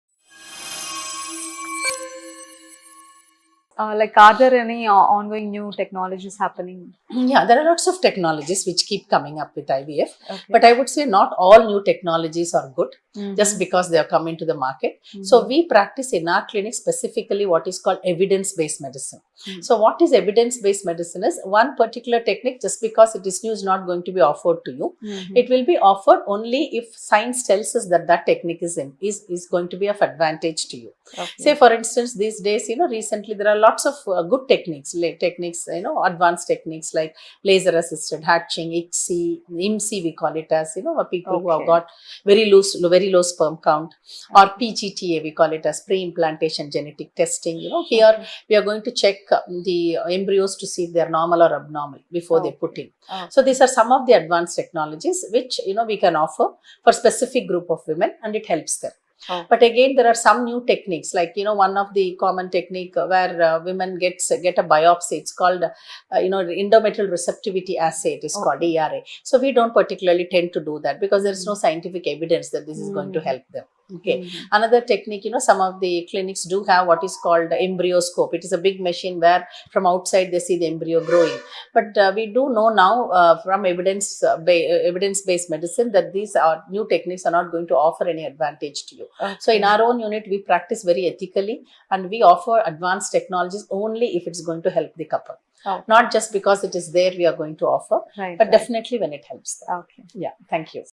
Revolutionizing Reproduction: Exploring New Technologies in IVF
In this exciting video, we take a deep dive into the world of "New Technologies in IVF." Join us as Dr. S. Vyjayanthi, a distinguished expert in Reproductive Medicine & Surgery, MD, DGO, DNB, MRCOG, MSC (Embryology UK), CCT (UK), Director and Fertility Specialist at MotherToBe, unveils the latest advancements shaping the future of assisted reproduction.

🔬 In this video, you'll explore:

🌐 Cutting-edge technologies transforming the IVF landscape.
📊 Innovative approaches to embryo selection and genetic screening.
🔍 Advancements in fertility preservation and egg freezing techniques.
🤖 The role of artificial intelligence and automation in IVF.
🌟 How these technologies improve success rates and patient experience.

Whether you're on a fertility journey, curious about the future of reproductive medicine, or interested in staying at the forefront of IVF innovations, this video is a must-watch.

🏥 Mothertobe Fertility Near Shilparamam Madhapur Hyderabad

Stay informed and inspired as we unveil the groundbreaking new technologies shaping the future of IVF. Join us on this journey of innovation!